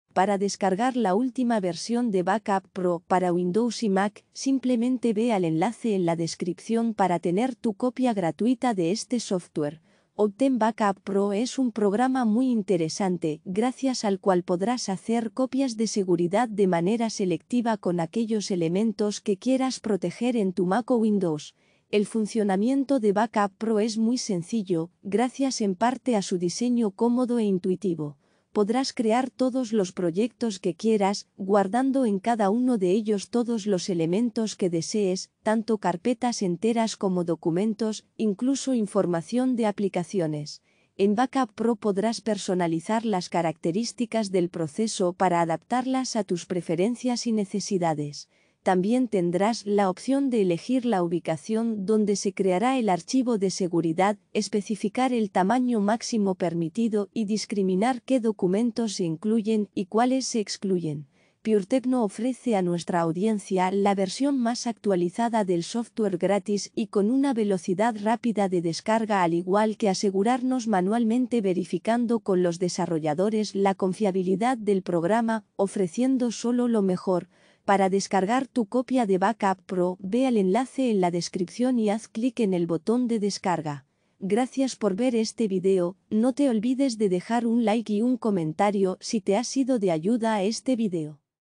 Descargar Backup Pro para Windows y Mac última versión
Get Backup Pro es un programa intrigante que le permite hacer copias de seguridad selectivas de los elementos de su Mac que quiere proteger. Si busca una herramienta de estilo, estás de suerte porque este programa es una gran opción.

La operación de Get Backup Pro es muy sencilla, gracias a su diseño cómodo e intuitivo. Puede crear tantos proyectos como quieras, almacenar todos los elementos que quieras en cada uno de ellos, incluyendo todas las carpetas y documentos, así como la información de la aplicación. Los proyectos que ya no quieres mantener pueden archivarse, duplicarse o sincronizarse de forma igualmente sencilla.

Puede adaptar las funciones del proceso en Get Backup Pro a sus preferencias y necesidades. También podrá especificar la ubicación en la que se creará el archivo de seguridad, el tamaño máximo del archivo y qué documentos se incluirán y qué se excluirán. También puede programar una copia de seguridad automática y su frecuencia.

Get Backup Pro es, en resumen, una herramienta bien diseñada y bien executada que le ayudará a mejorar la seguridad de su Mac en caso de fallos del sistema o pérdidas de datos.